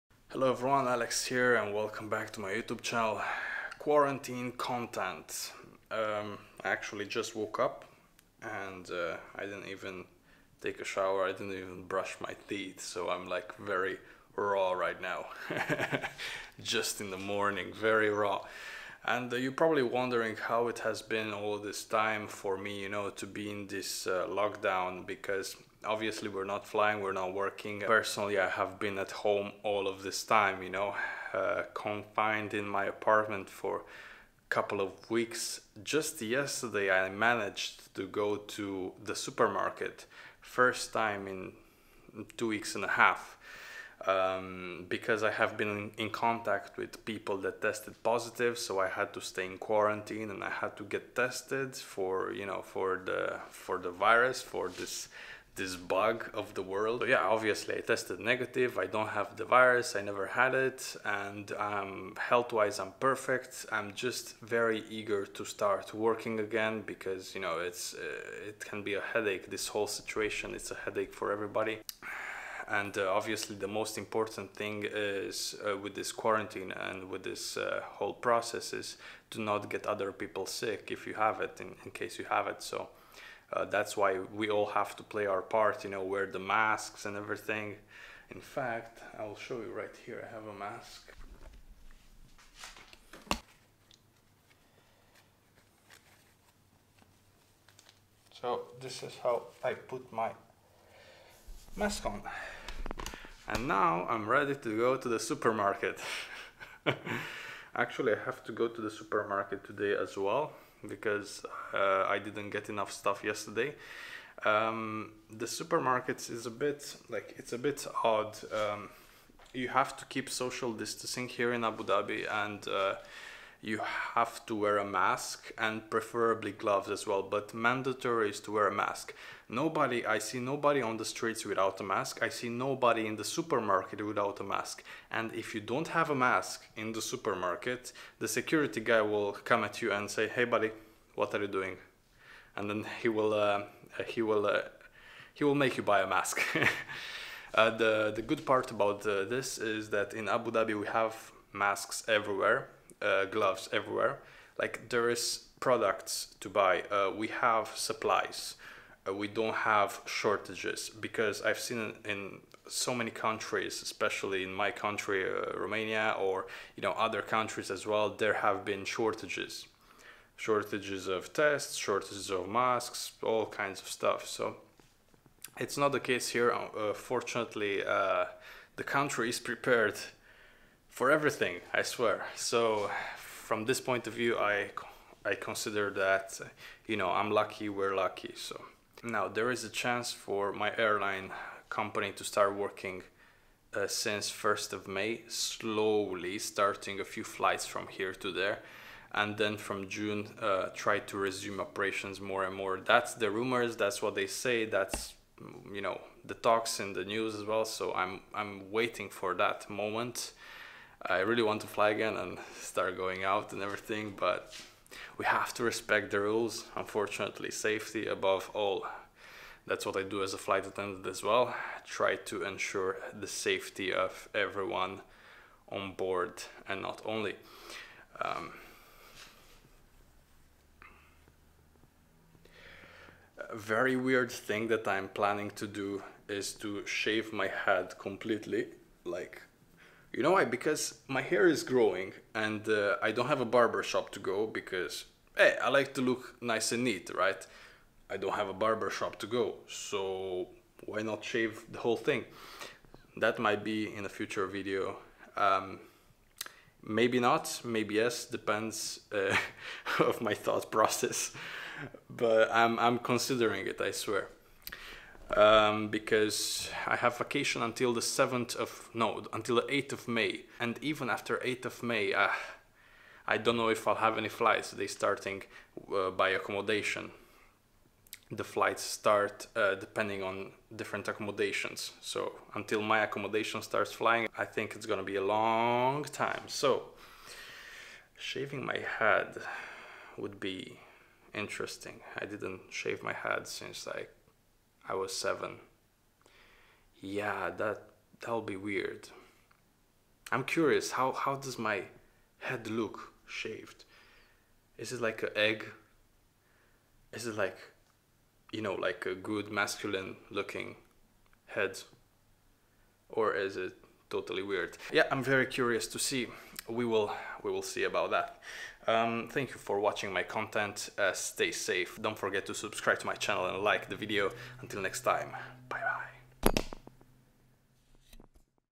Keeping the MASK ON
Stepping out of my apartment is a rare thing!

BTC wallet:
3Pi2r2w6kRmgk49FBVejSQabNgsen5yZHg